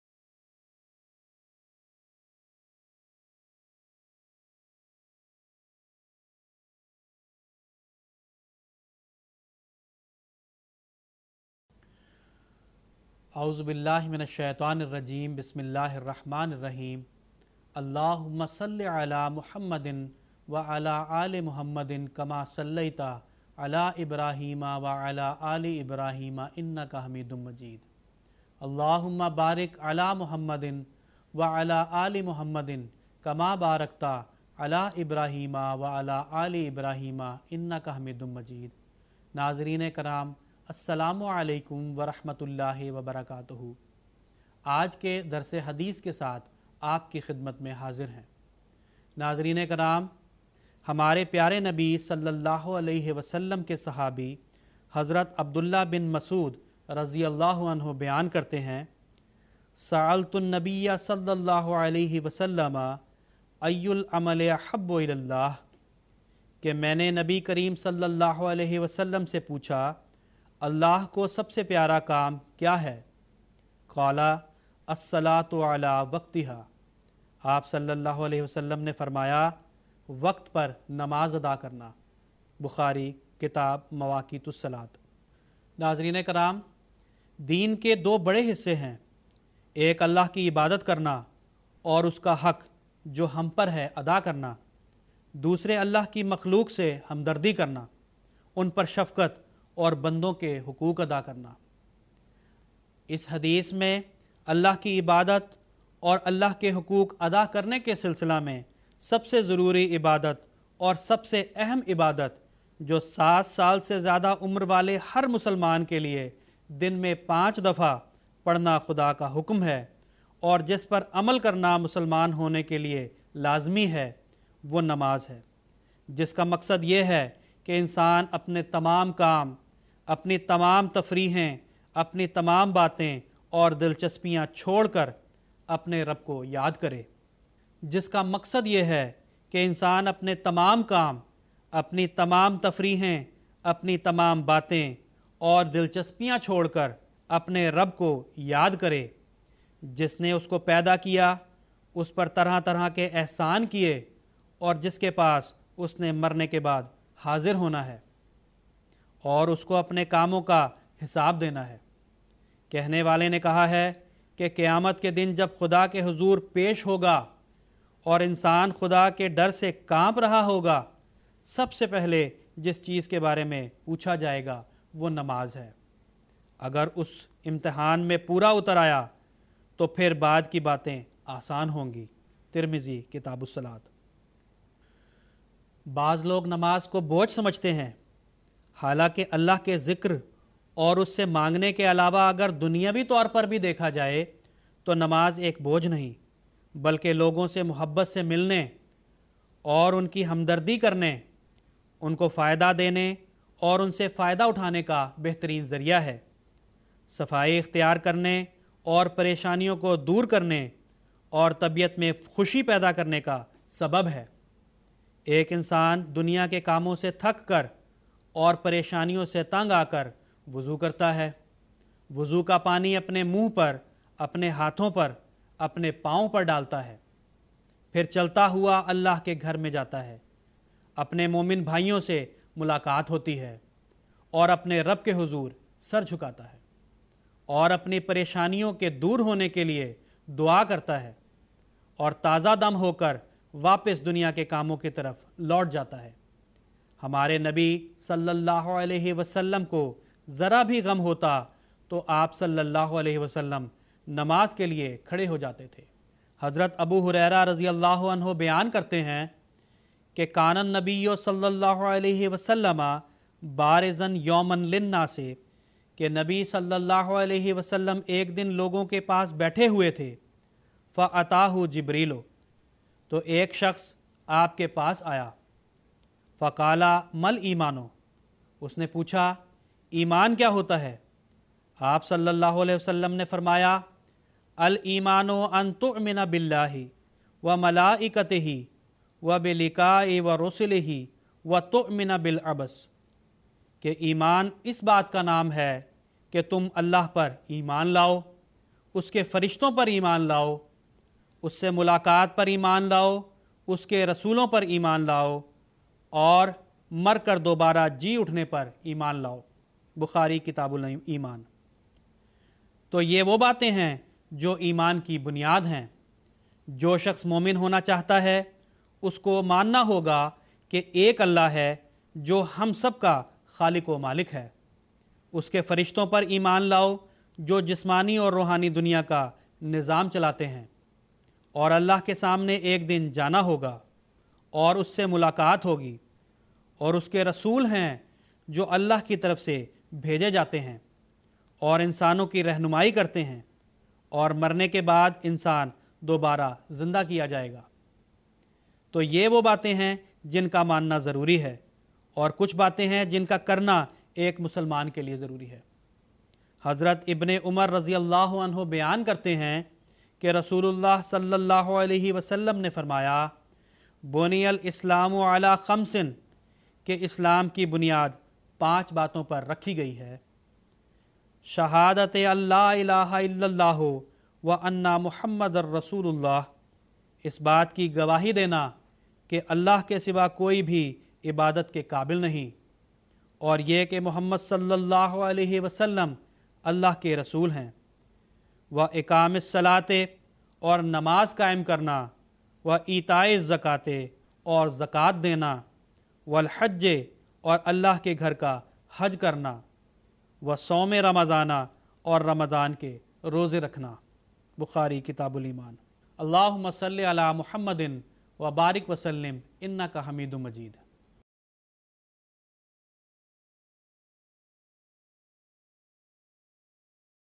Dars-e-Hadith - Offering prayers on time
Nasr MinAllah presents selected sayings of the Holy Prophet Muhammad (saw) on the topic of learning and teaching the Holy Qur'an. Presentation MTA studios India.